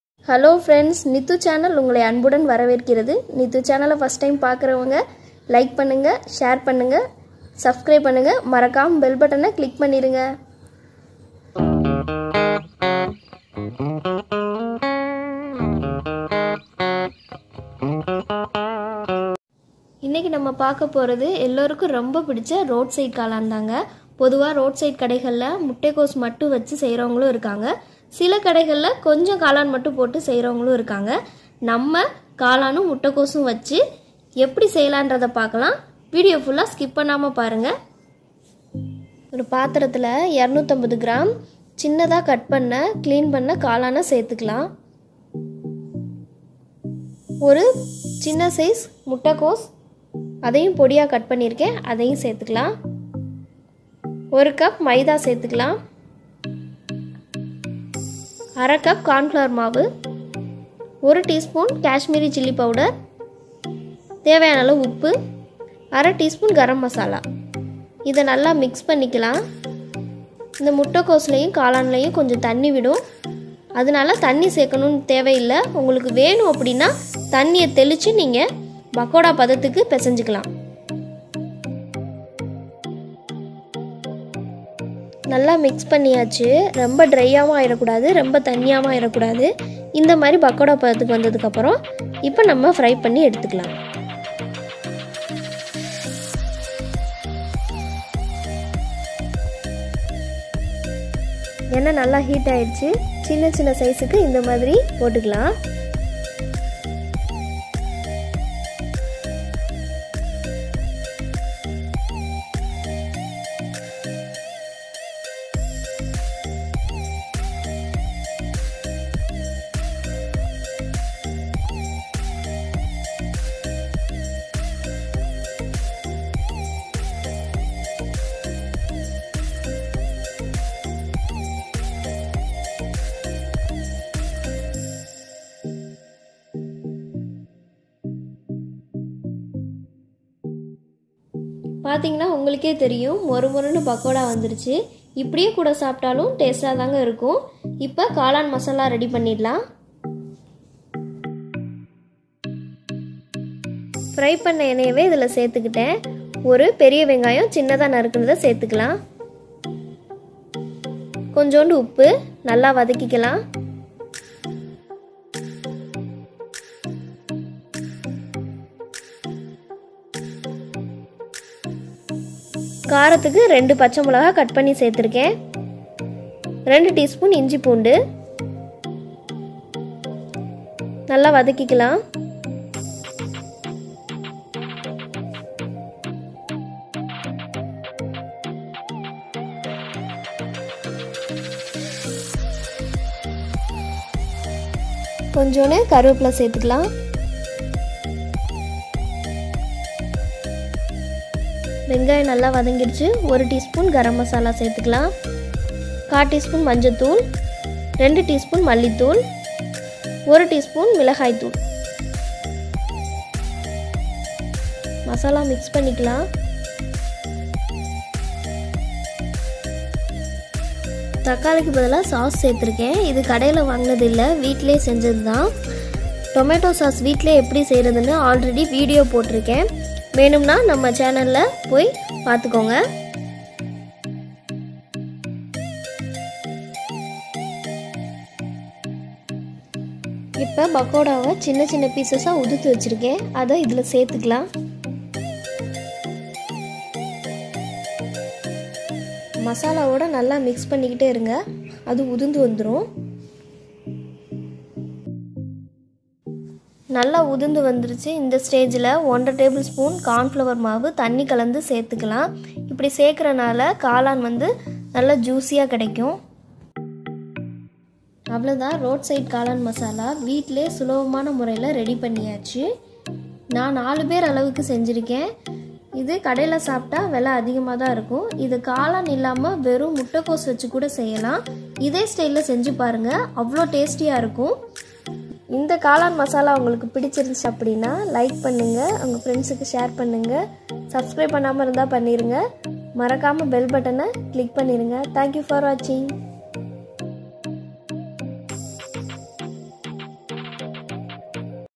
Roadside Kaalan Recipe in Tamil | Kaalan Masala | Home Made Kaalan Recipe| Most Famous Street Food
Hope you enjoy these recipe,
Just try it and leave your comment how they came out for you

Always have a healthy food which is Made at Home 🙂